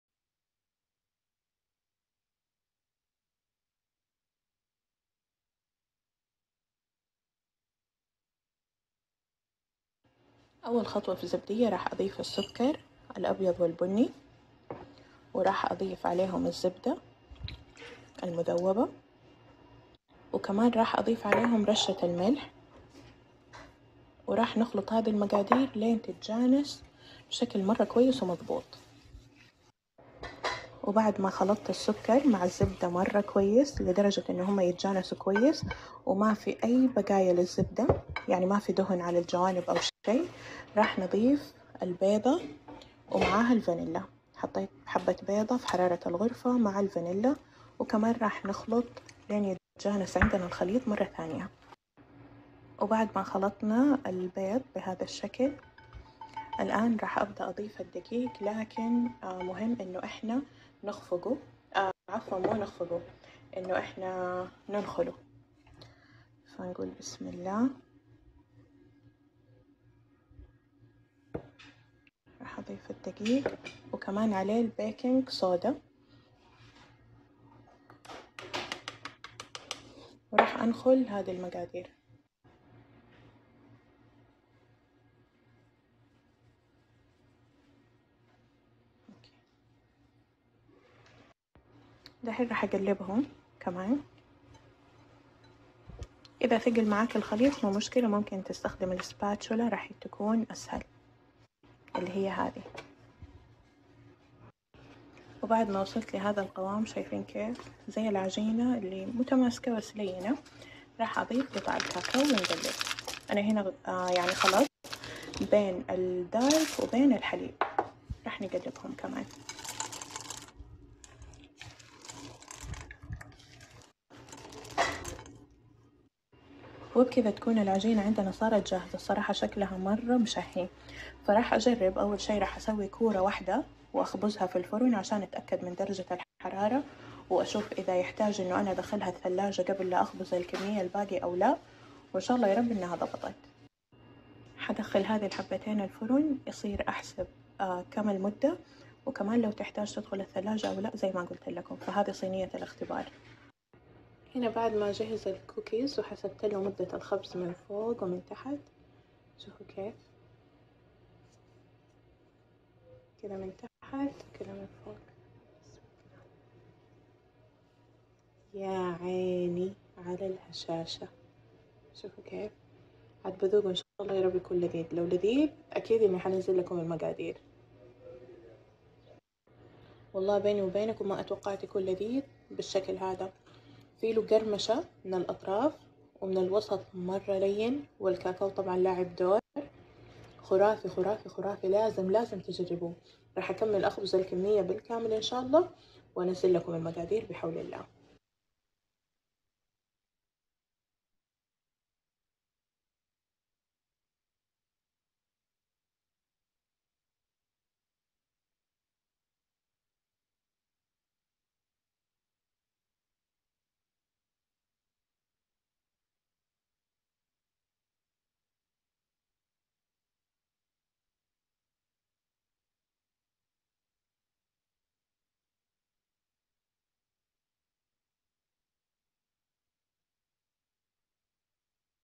كوكيز الزبدة الذايبة 🍪😋 من الذ انواع الكوكيز ترف ولذيذ وذايب 👌✨ هند حبيب
كوكيز الزبدة الذايبة 🍪😋 من الذ انواع الكوكيز ترف ولذيذ وذايب 👌✨ سناب هند حبيب ...
لاتنسوها من دعواتكم
* المقادير //
اصبع زبده ١٠٠ جرام مذوب ونخليه على جنب تدفى
ثلاث ارباع كوب سكر بني طري
نص كوب سكر ابيض ناعم
حبه بيضه في حراره الغرفه مع ملعقه صغيره فانيلا
كوب وثلاث ارباع دقيق ابيض
ملعقه صغيره ملح
ملعقه صغيره بيكربونات الصوديوم
كوب وربع كاكاو بلجيكي انا خلطت بالحليب والدارك
والبدائل(جالكسي ساده + جالكسي دارك او كاكاو كادبوري الموف الذ من الجالكسي في الكوكيز👌🏽)
* الطريقة //
١- اول شي نخلط السكر البني والابيض مع الزبده كويس كويس لين تتجانس الزبده معاهم
٢- نضيف حبه البيضه مع الفانيلا ومهم جداً انها تكون في حراره الغرفه 👌🏽 ونخلطها مع السكر لين تتجانس
٣- بعد كذا ننخل الدقيق ونضيفه مع بيكربونات الصوديوم على الخليط ونعجن تتكون لك عجينه قوامها متماسك زي الفيديو
٤- بعدها نضيف الكاكاو ونقلب ونحط الخليط في الثلاجه ربع ساعه
٥- نسوي صينيه اختبار لحبتين كوكيز ليش(عشان تتاكدي من درجه حراره فرنك وماتخرب الكميه كلها وحطي مؤقت في جوالك
٦- ندخل الكوكيز في فرن حار مسبق على ١٨٠ درجه مؤيه او ٣٥٠ فهرنهايت من تحت بالضبط٧ دقايق ودقيقه بس من فوق ونطلعها ونخليها ١٠ دقايق في الصينيه وبعدها نطلعها وبالعافيه😋😋😋
جربوها مع الايسكريم 🍦
مشاهدة ممتعة ،،